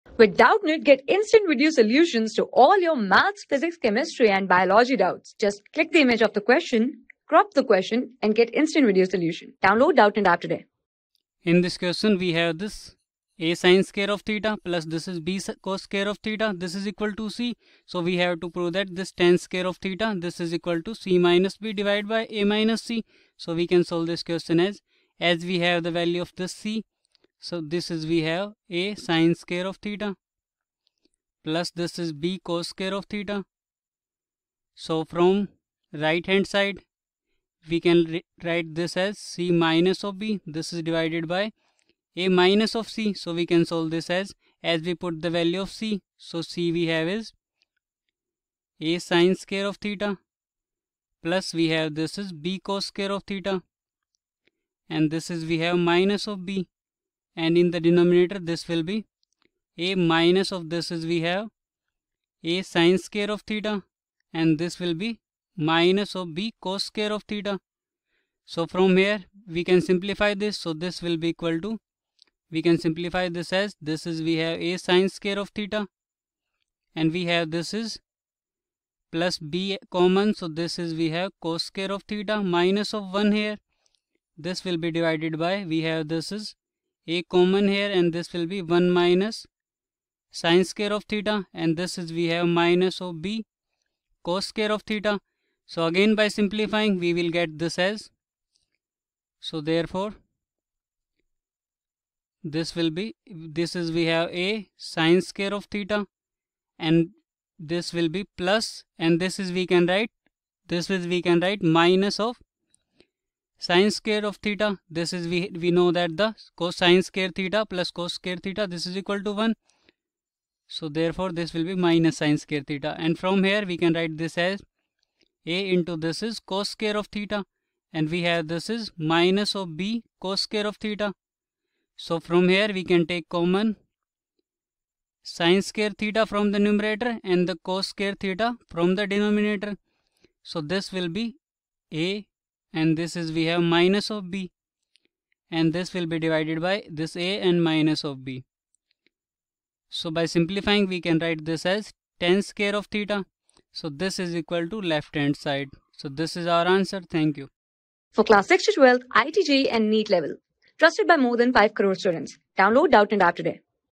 If asin^(2)theta+bcos^(2)theta=c, then prove that tan^(2)theta=(c-b)/(a-c) | 11 | TRIGNOMETRIC F...
If asin^(2)theta+bcos^(2)theta=c, then prove that tan^(2)theta=(c-b)/(a-c)
Class: 11
Subject: MATHS
Chapter: TRIGNOMETRIC FUNCTIONS
Board:CBSE

👉 Have Any Query? Ask Us.